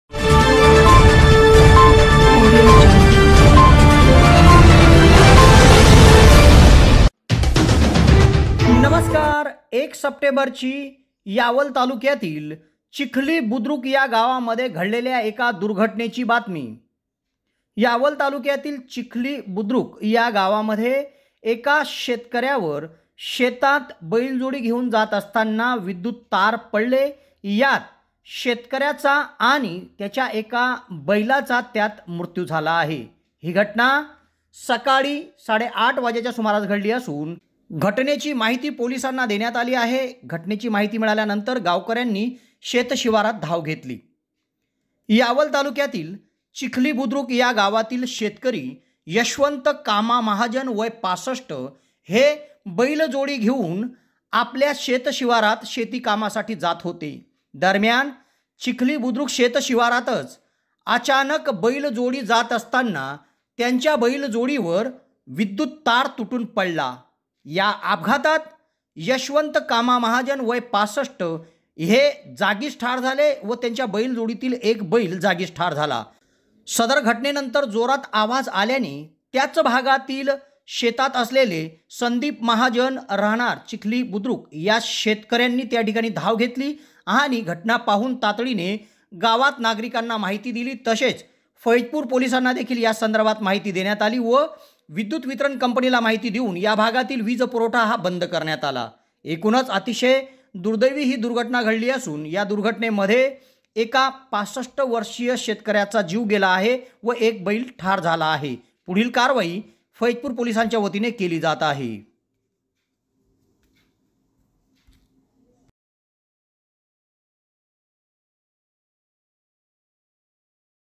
01 September 2022 यावल तालुक्यातील चिखली बुद्रुक गावात बैलगाडीवर विजेचे तार पडले.
यावल तालुक्यातील चिखली बुद्रुक गावात बैलगाडीवर विजेचे तार पडले.